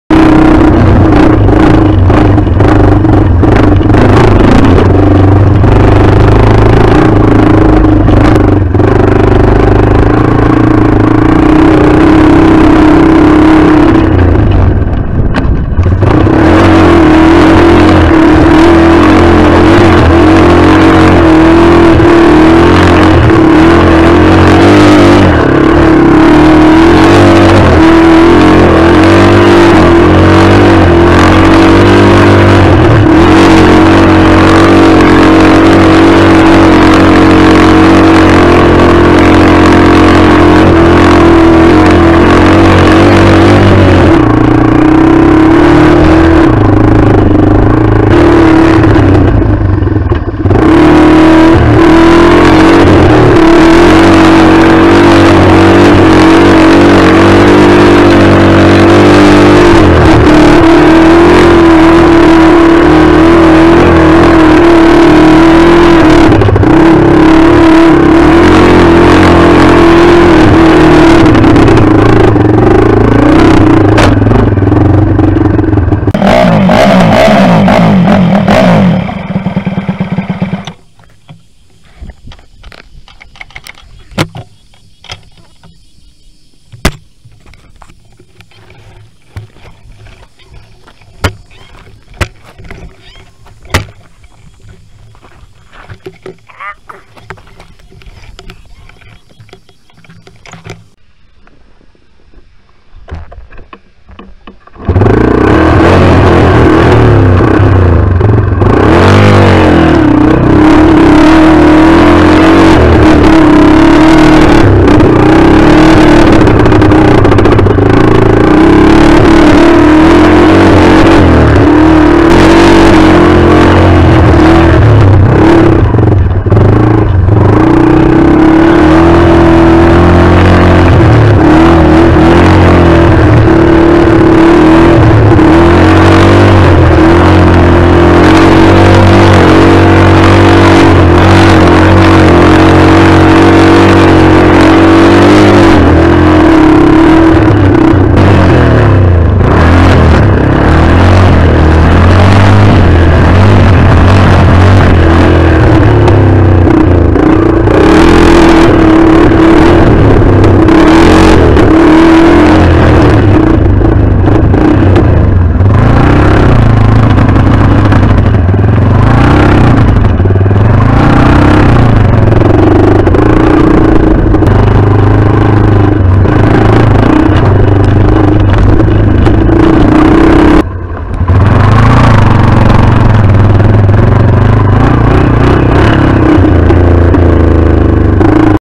AG 200 Jumps, Drifts and Hill Climbs
riding up in munibung hill on a 1992 Yamaha ag200